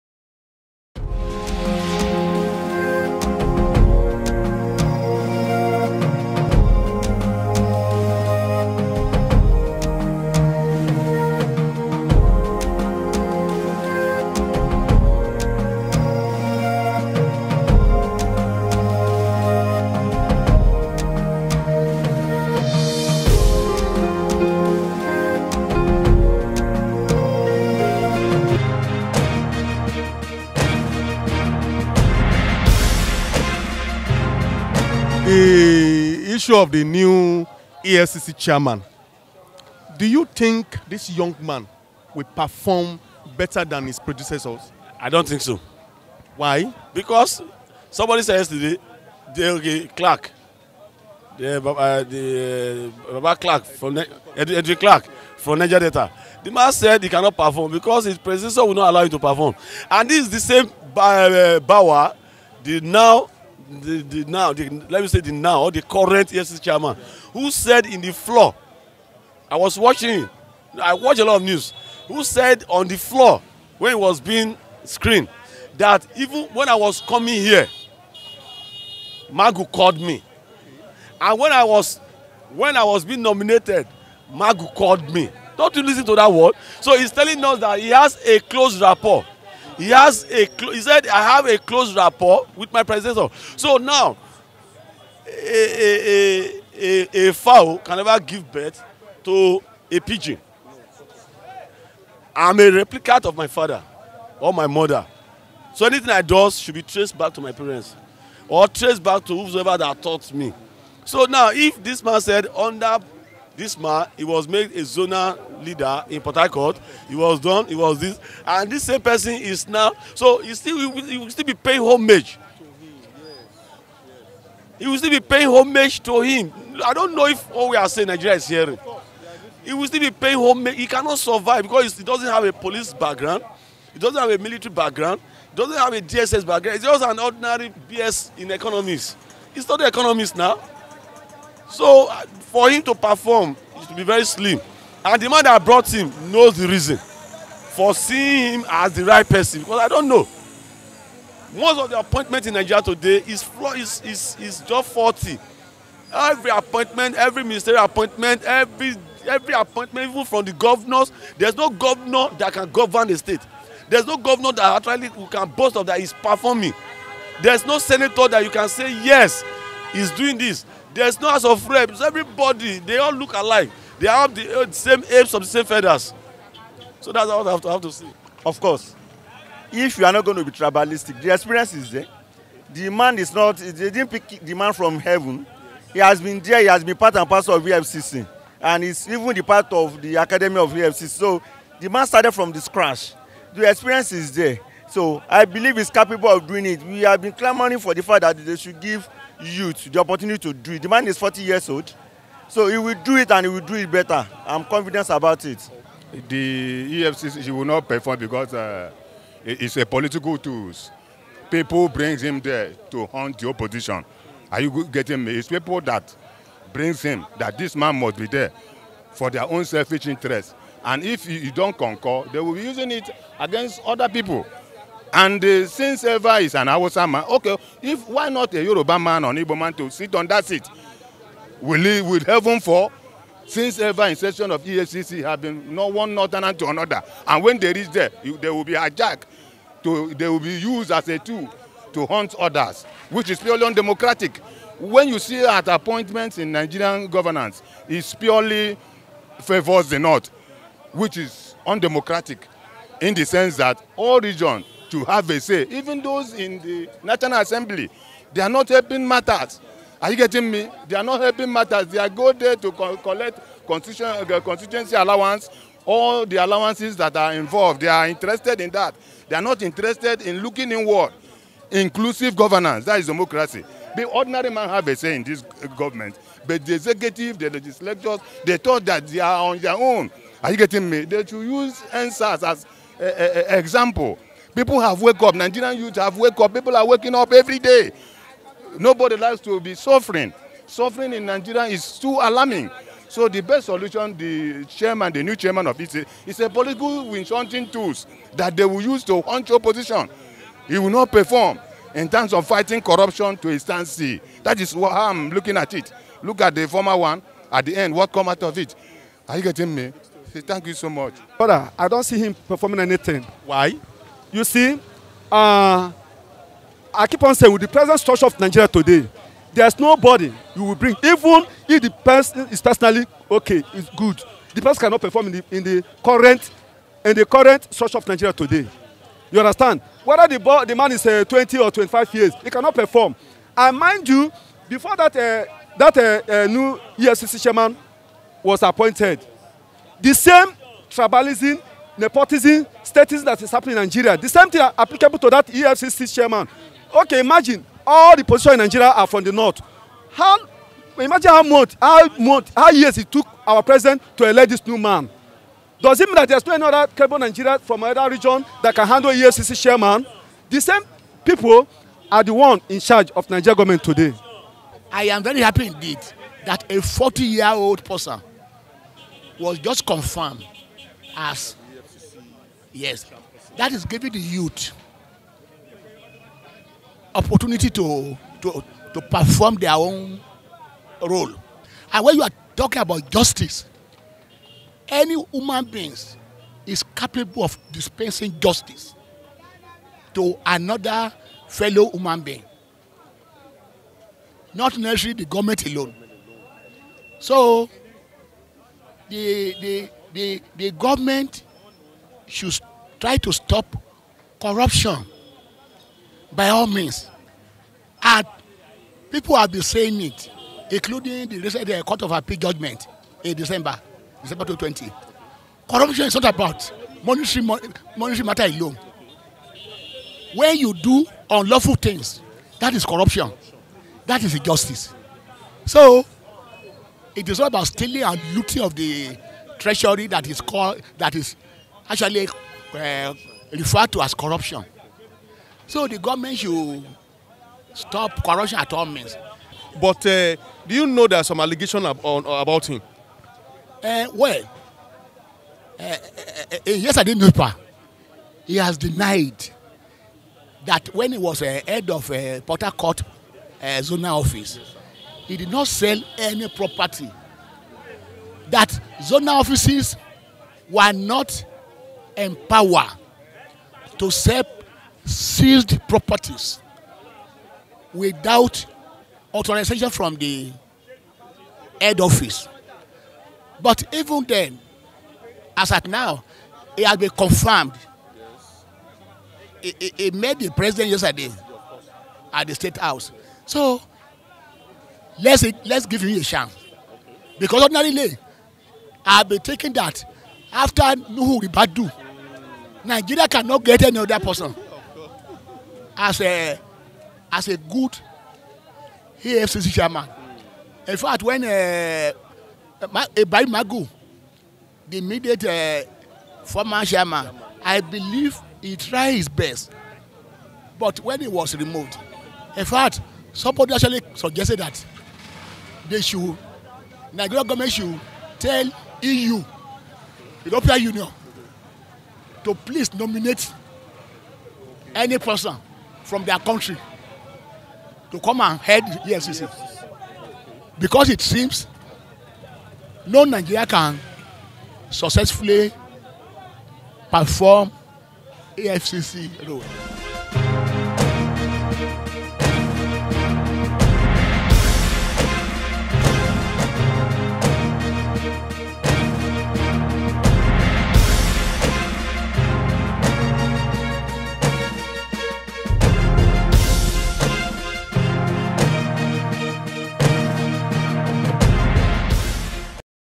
Nigerians Share their thought on New EFCC Boss, AbdulRasheed Bawa
Nigerians share their thought on the New EFCC Boss, AbdulRasheed